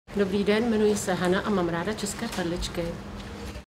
My profession: Hana
"Dobrý den, jmenuji se Hana, a mám ráda české perličky"

Tradiční české perličky - Traditional Czech Beads
perličky, mačkané perle - seed beads, presed beads
unikátní dvoudírkové perličky - unique two-hole seed beads - PRECIOSA Twin™
PRECIOSA ORNELA navazuje na jedinečný fenomén tradice českého sklářství.
PRECIOSA ORNELA is a part of the unique phenomenon of Czech glassmaking.